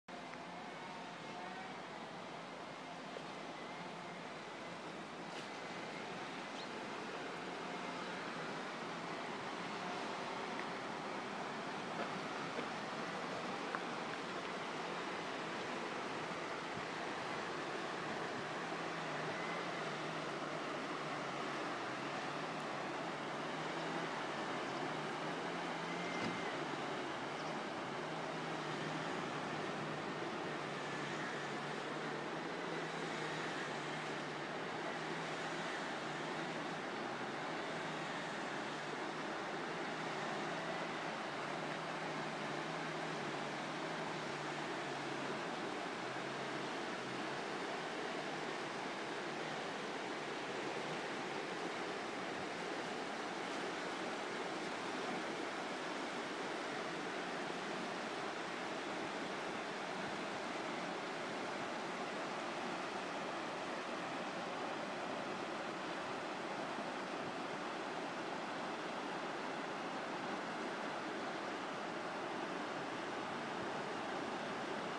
Ardfinnan24/10/11
A day after heavy wind and rain in the area ,also showing the flood defences at bretts yard